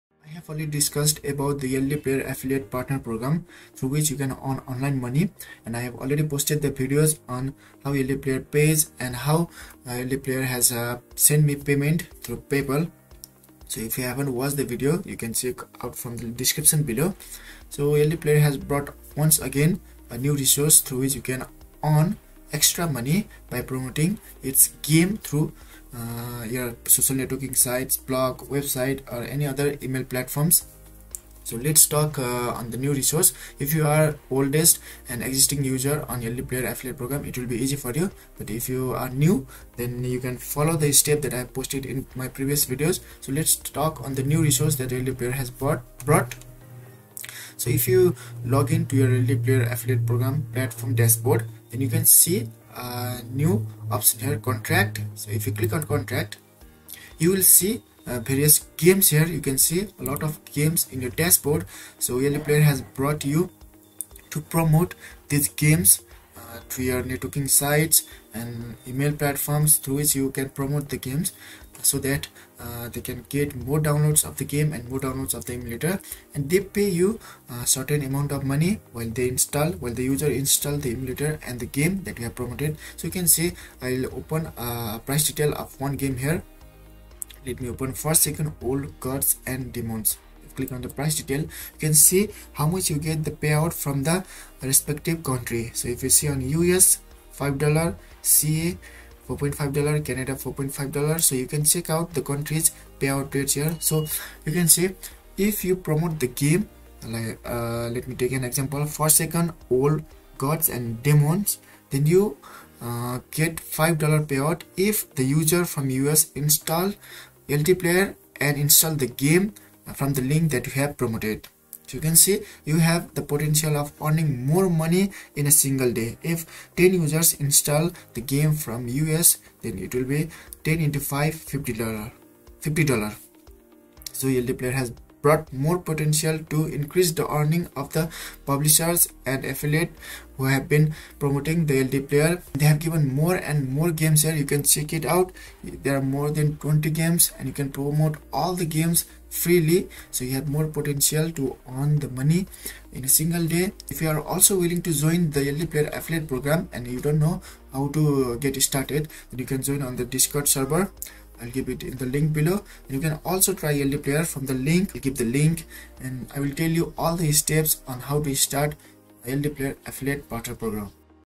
Make $2-$5 Playing Games Sharing Games || How to make Money Online || Pubg Mobile, Mobile Legends
Earn $2-$5 Playing Games Sharing Games || How to Earn Money Online || Pubg Mobile, Mobile Legends
Here you will learn how to earn some pennies just by sharing the games to others to play.